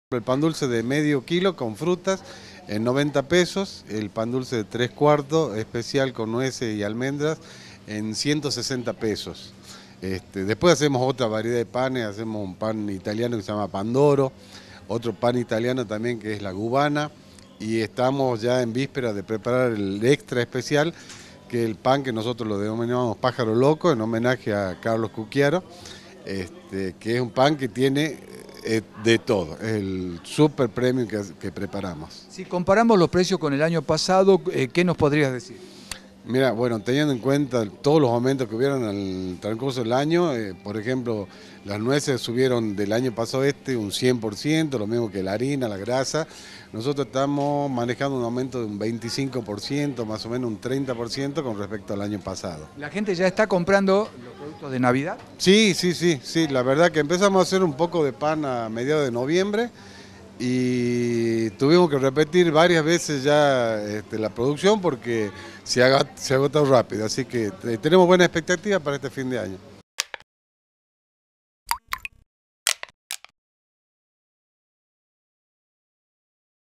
Pan dulce, turrón y budines este año tendrán un incremento de más del 20%
Se acercan las fiestas de fin de año y los bolsillos empiezan a sentir las subas.